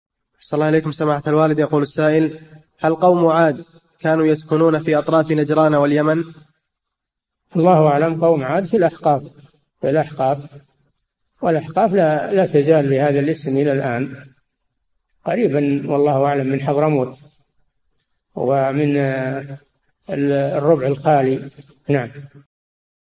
[672- 4717] أين كان يسكن قوم عاد؟ - الشيخ صالح الفوزان
"[4717-672] هل قوم عاد كانوا يسكنون في أطراف نجران واليمن؟
الجَوَابُ: الله أعلم قوم عاد في الأحقاف، والأحقاف لا تزال بهذا الاسم إلى الآن قريبًا والله أعلم من حضرموت، ومن الربع الخالي.

للاشتراك في قناة الشيخ صالح الفوزان
فتاوى المجالس في تفسير المفصل

للاستفسار والإقتراحات والدعم
رئيس الفريق
لمشروع كبار العلماء